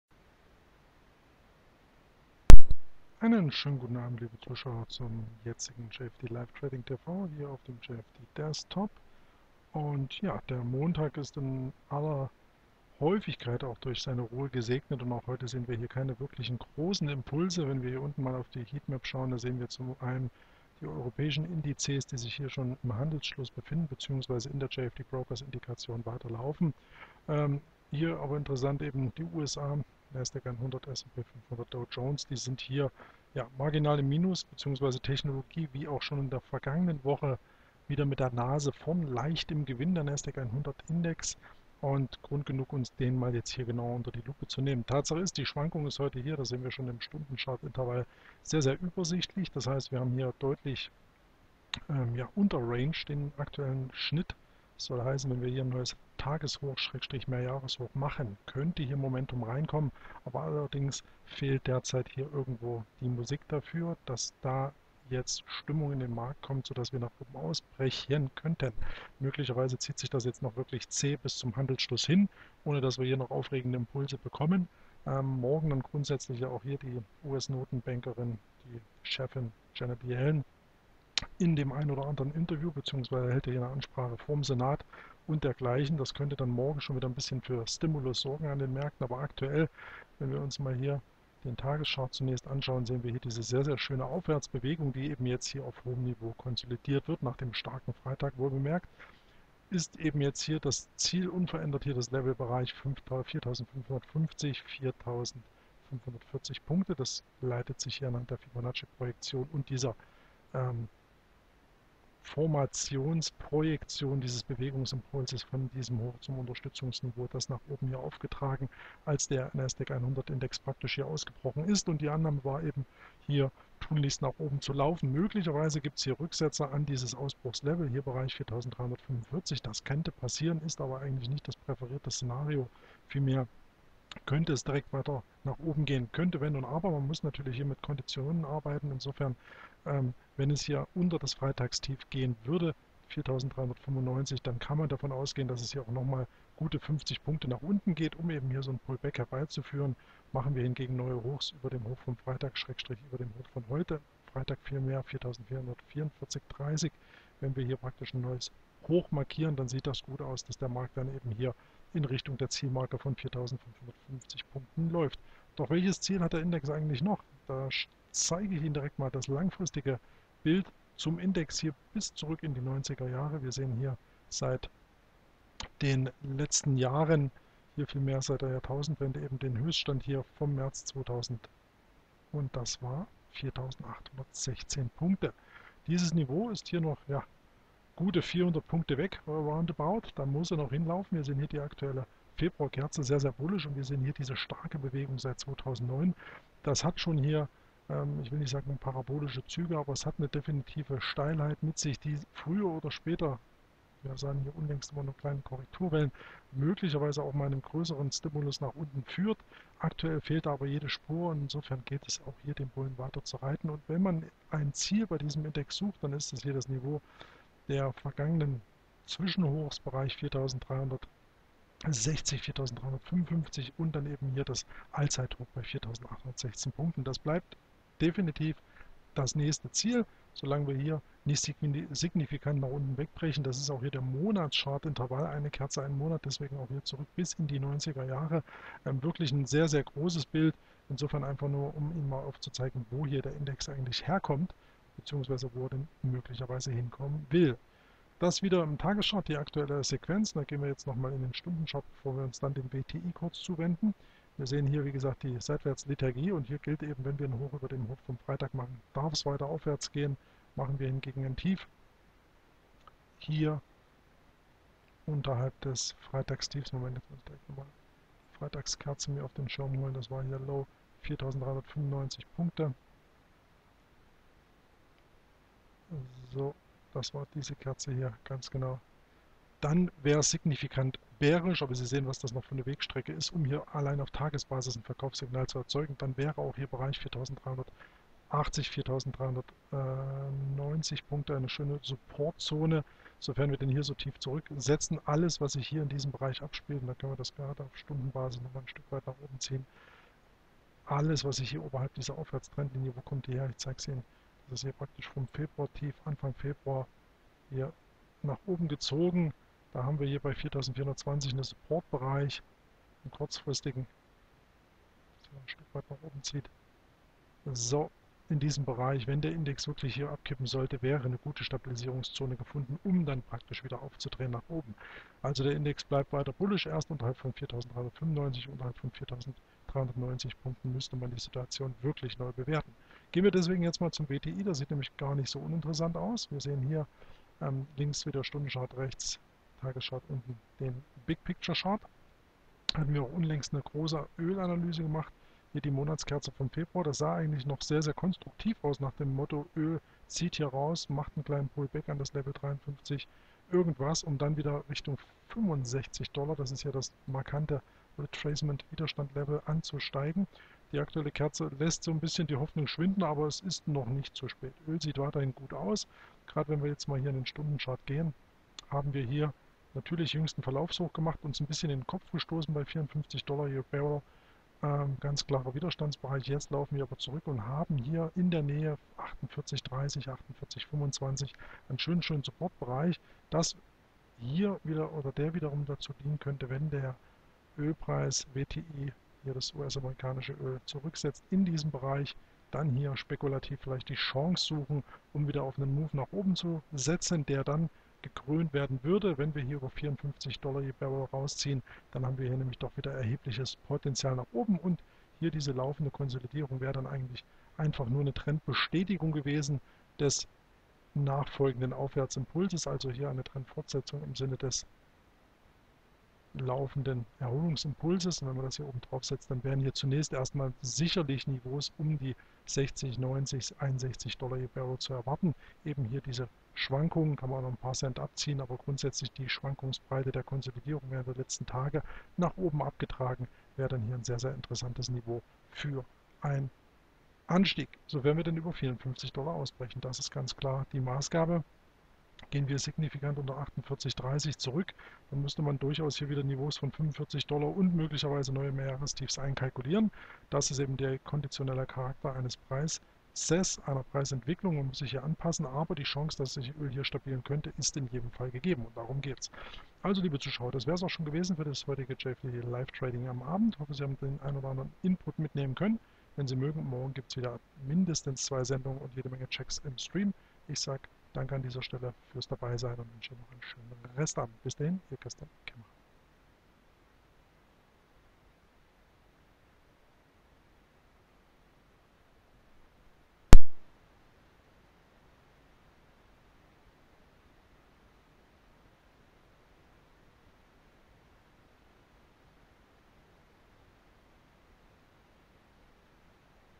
JFD Live Trading TV - Nasdaq 100 Index und WTI-Öl
Zum heutigen Wochenauftakt zeigen sich die US Indizes sehr verhalten. Auf welche Marken man dennoch achten sollte sehen Sie hier. WTI rundet die Beschau mit einem interessanten Setup ab.